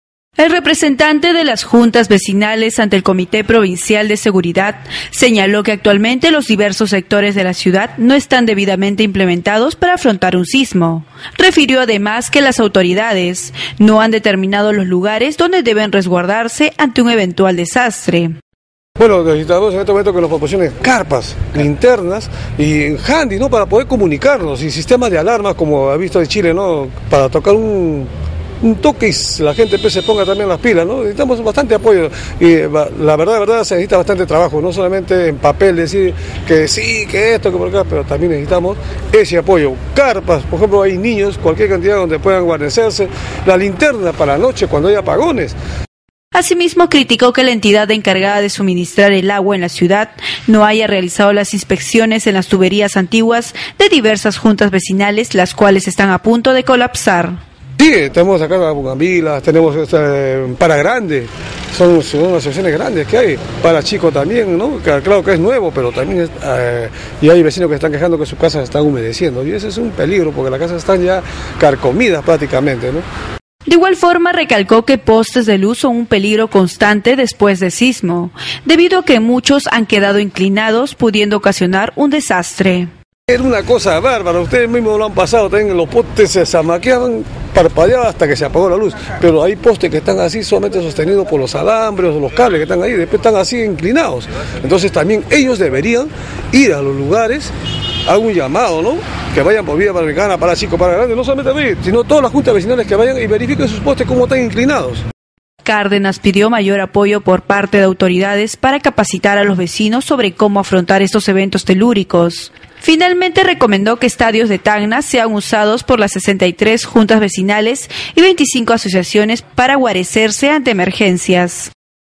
JUNTAS VECINALES NO ESTÁN IMPLEMENTADAS PARA AFRONTAR SISMOS
El Representante de las Juntas Vecinales ante el Comité Provincial de Seguridad señalo que actualmente los diversos sectores de la ciudad no están debidamente implementados para afrontar un sismo.